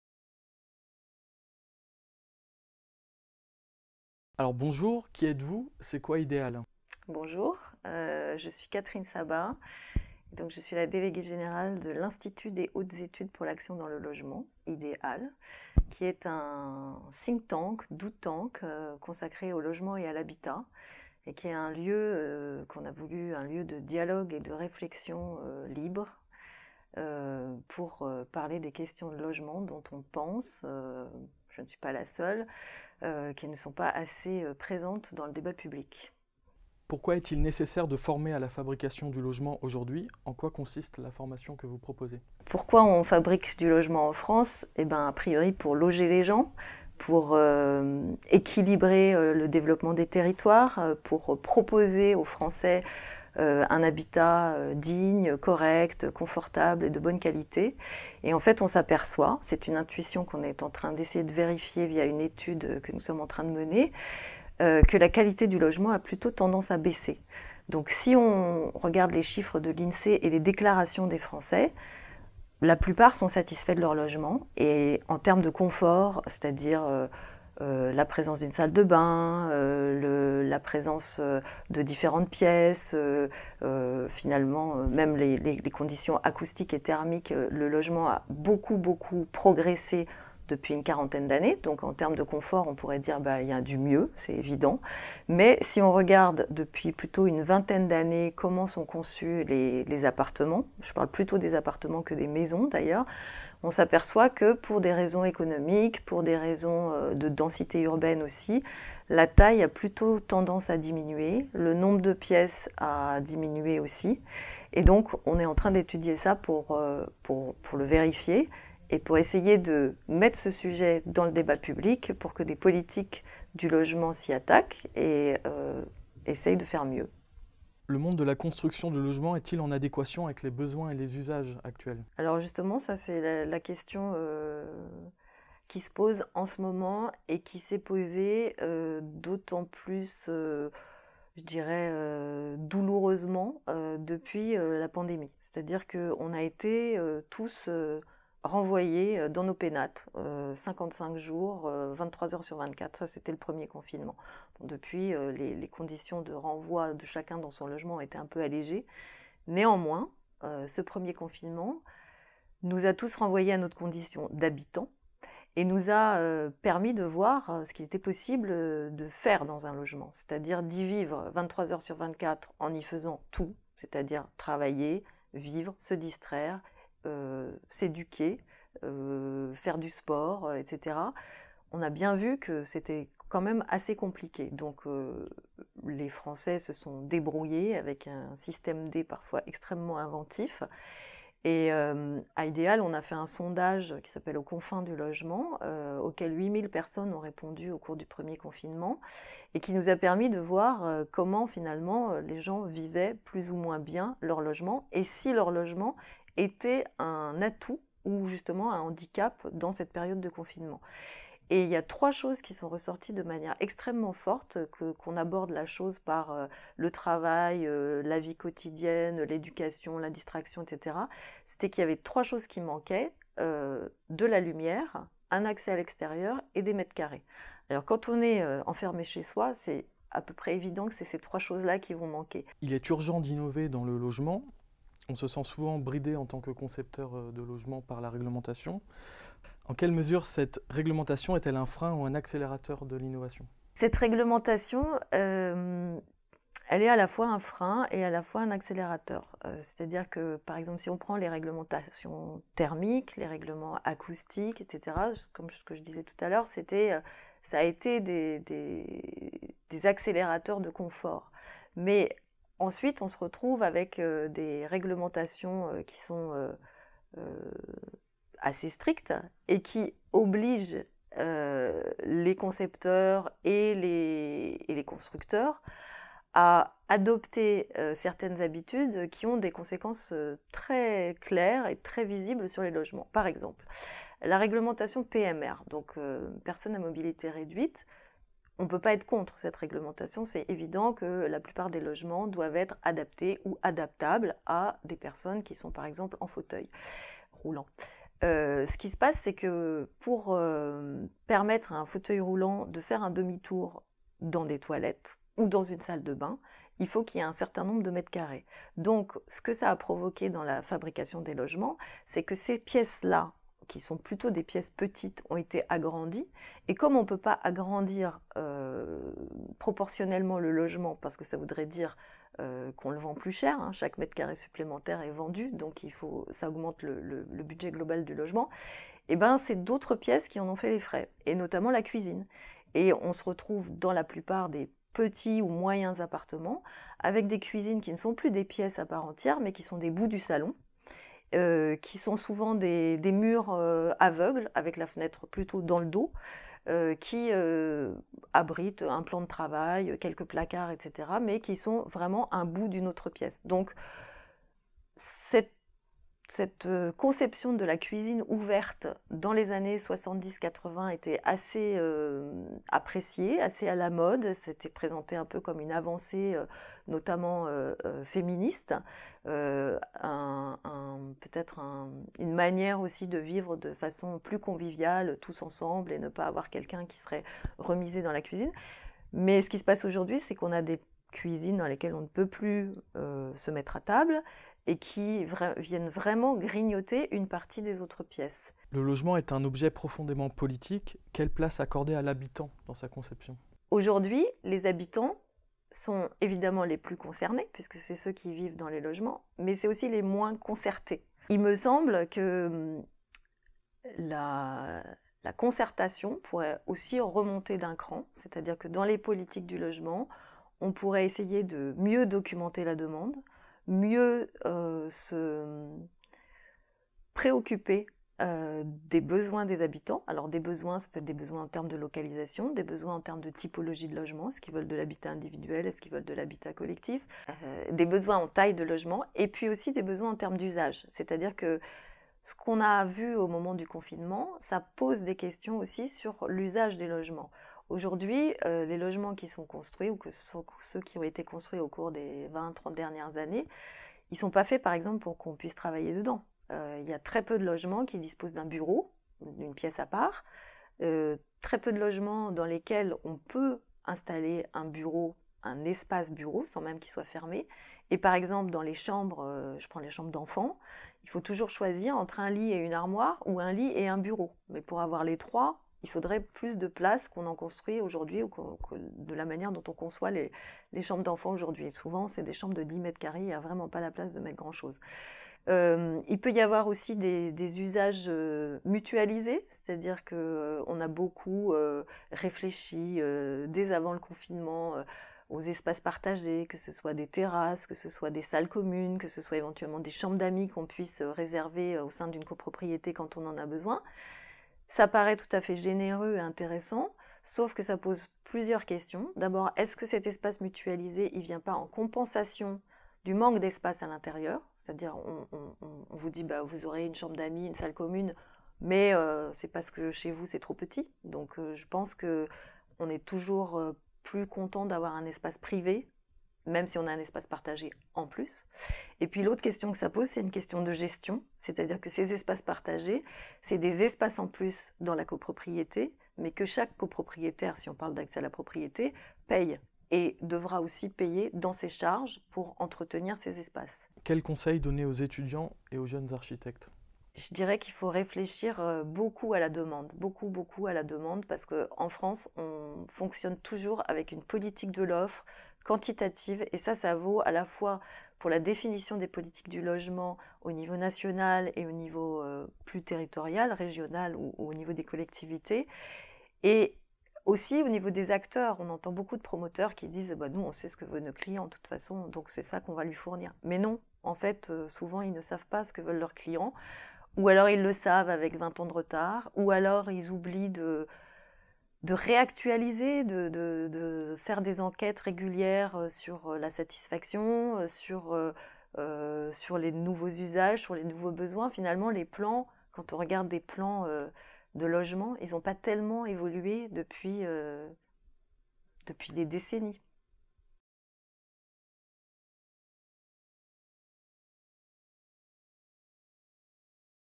Catherine Sabbah - IDHEAL / UNI
Nous rencontrons aujourd'hui Catherine Sabbah. Journaliste, elle se consacre maintenant au Think Tank, IDHEAL (l'institut des hautes études pour l'action dans le logement). En France, un constat est clair la qualité de nos logements tend à diminuer depuis les années 2000. Il est nécessaire de mieux former nos maîtrises d'ouvrage  pour tendre collectivement vers une amélioration de l'offre. Catherine Sabbah, nous donne un conseil pertinent et fort de sens, peut être devons nous s'attacher un peu plus à la demande, requestionner nos manières de faire, et s'adapter avec hardiesse à dessiner autrement.